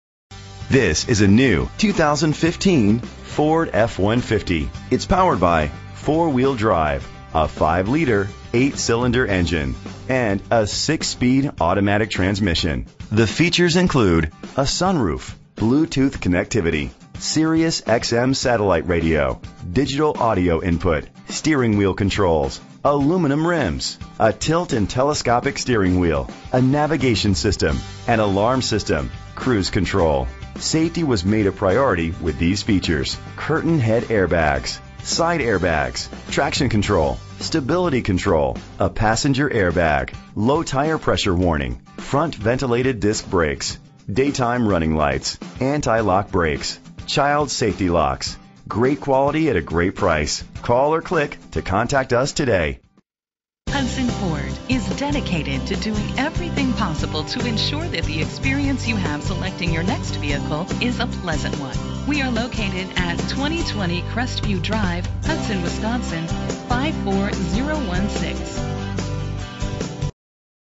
2015 Ford F-150 - Hudson WI
Phone:
Year: 2015
Make: Ford
Model: F-150
Trim: XLT
Engine: 5.0 liter 8 cylinder 32 valve
Transmission: 6-Speed Automatic
Color: Ruby Red Metallic Tinted Clearcoat
Mileage: 0
Address:
2020 Crestview Drive